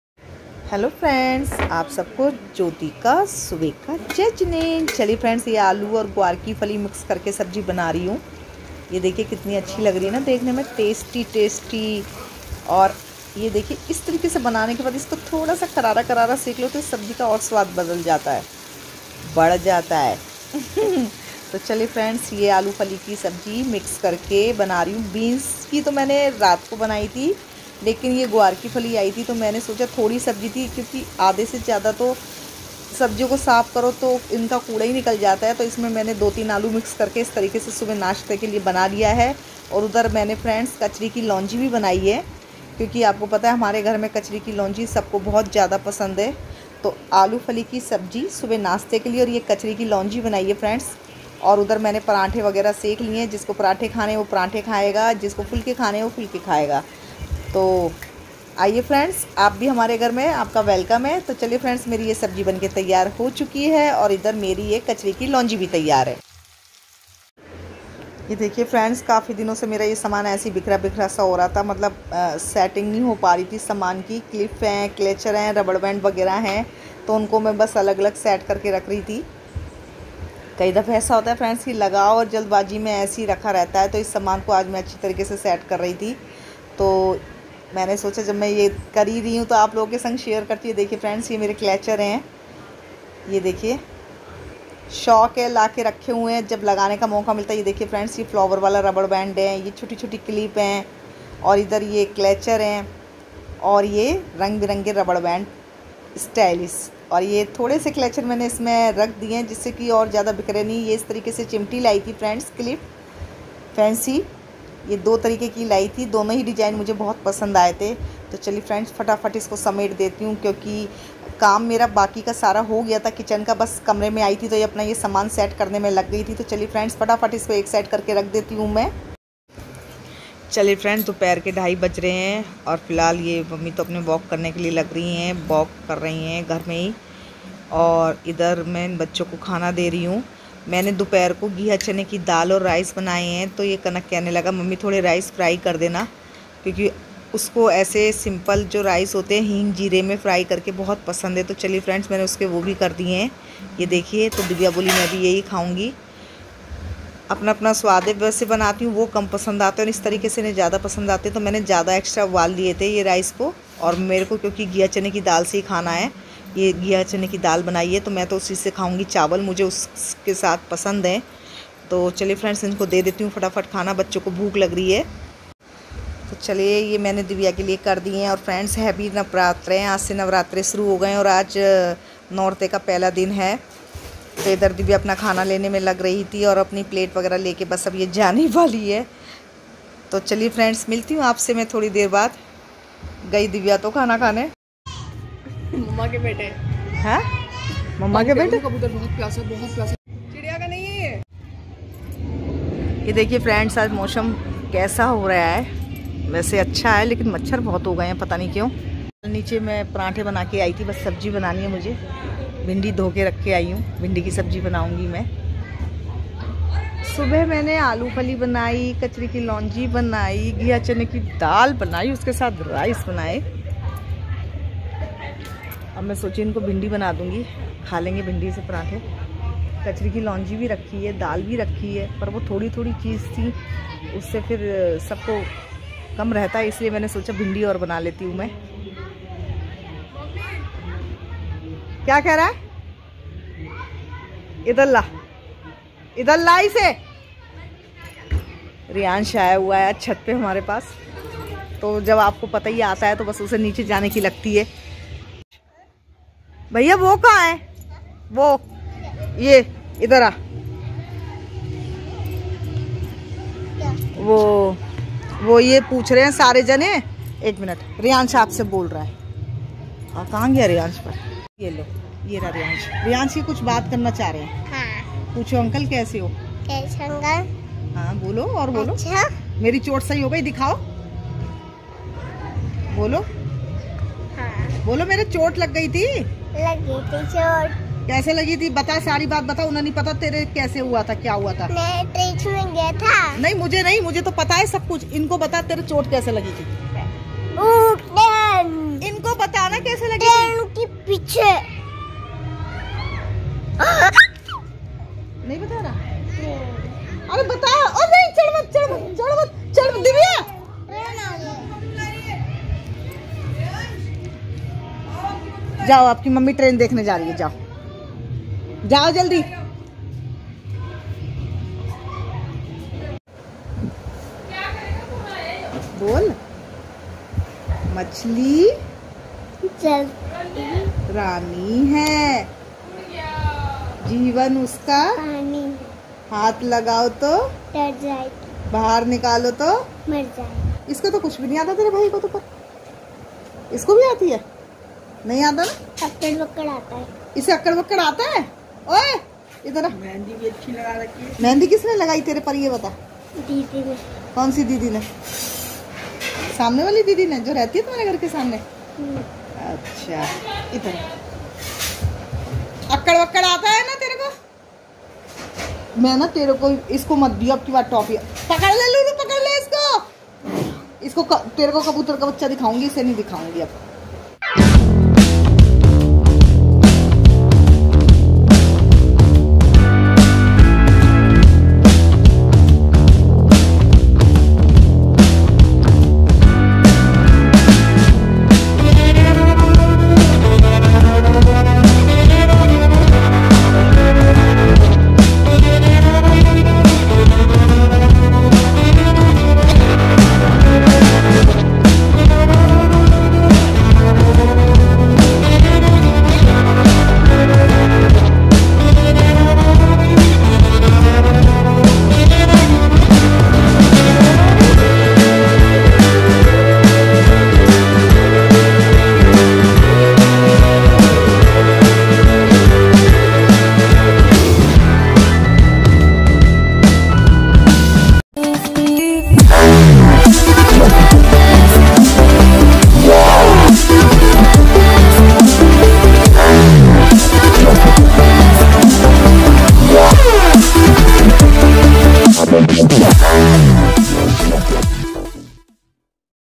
aap sabhi ko Jyoti Jain ki taraf se bahut bahut shubhkamnayen (Happy Navratri 🙏)
hello friends aaj maine Subah nashte Mein Aalu aur gwar ki fali Mix Karke sabji Banaye hai aur uske sath Maine kachri ki launji bhi Banai Hai dopahar Ke lunch Mein Maine ghiya chane ki daal aur rice banae Hai sham ko dinner Mein bhindi ki fry sabji Banai hai please friends Mere channel ko subscribe like share and comment Jarur Jarur Kijiye aapka bahut bahut dhanyvad